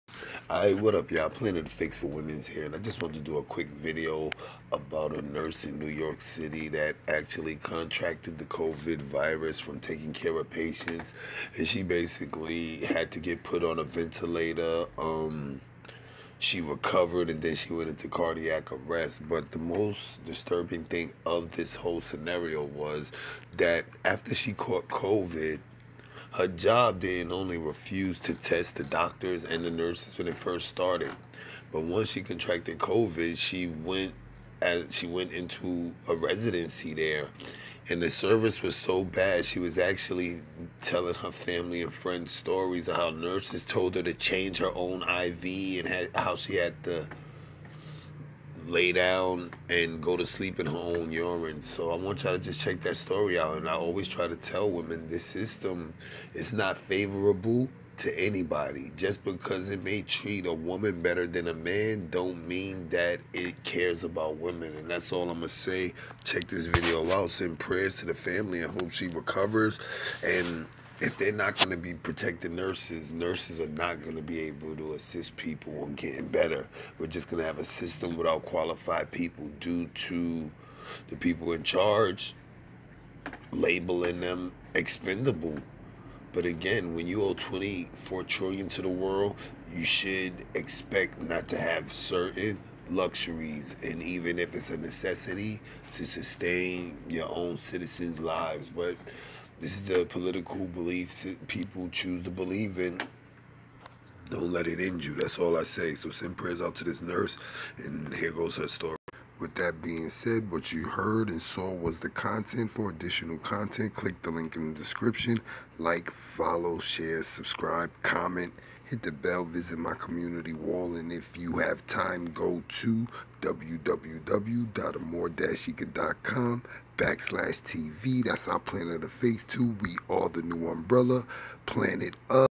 New York NURSE Rona tale and NYC Reopening for business
"The hospital would not test them - they're not testing the doctors, they're not testing the nurses even if they're sick," said Licin.